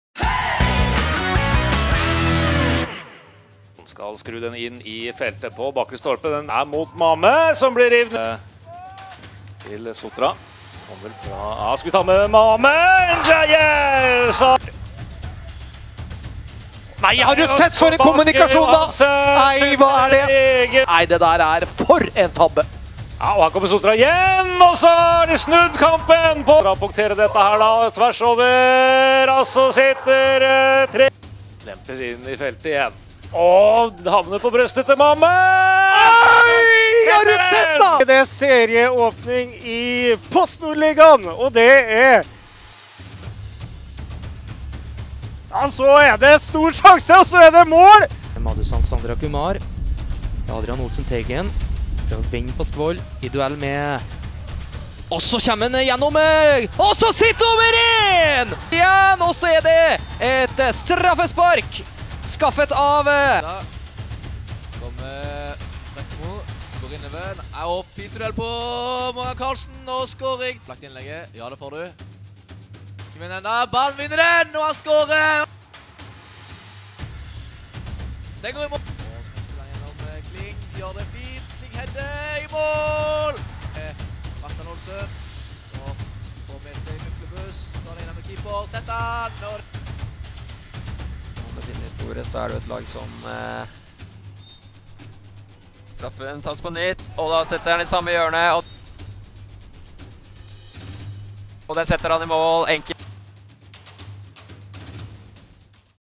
PostNord-ligaen Målshow 2020 - Avdeling 2 Runde 1
Alle målene fra serieåpningen i PostNord-ligaen avdeling 1. Enkelte kamper og mål kan mangle.